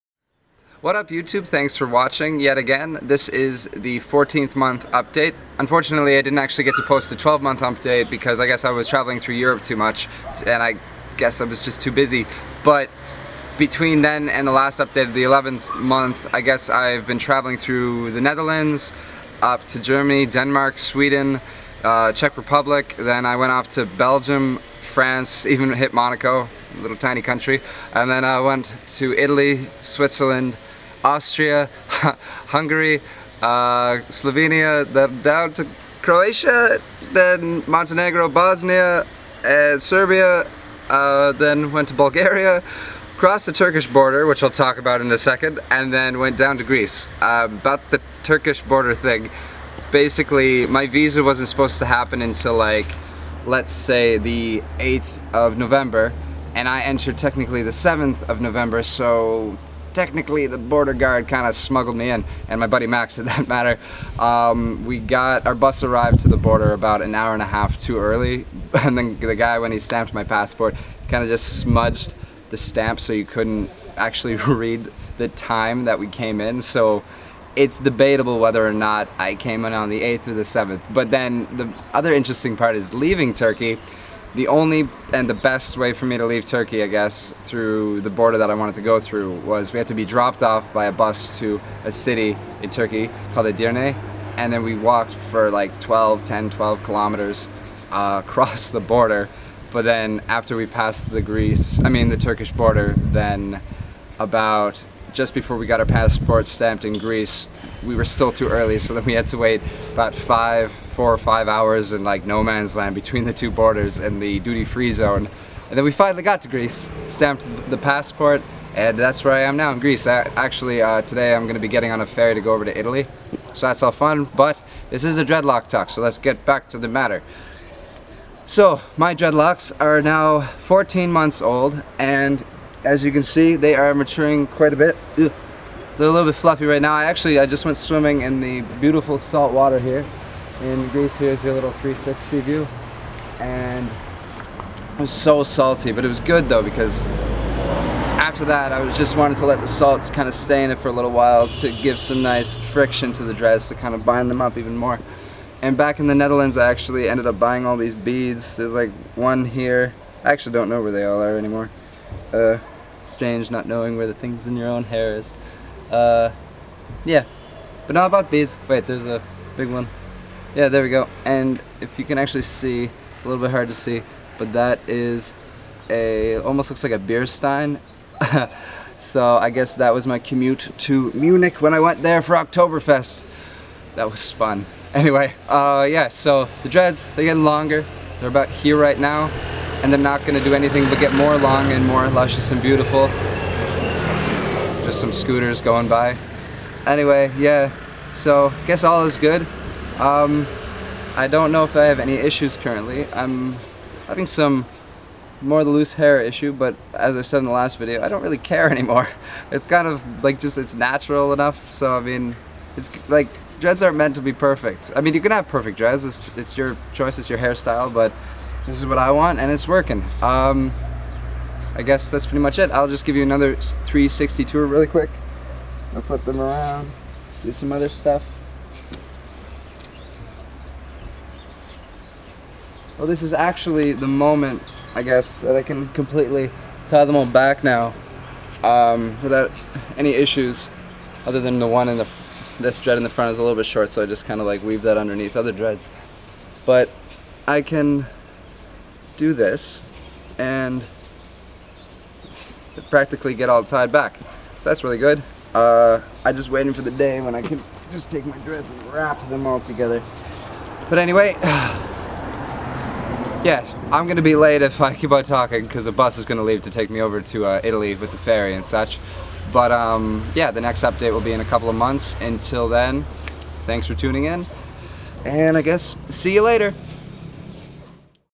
Month 14 - Dreadlocks
This video is showing the 14th month update on the Dreads.  Kind of highlighting my travels abroad as well.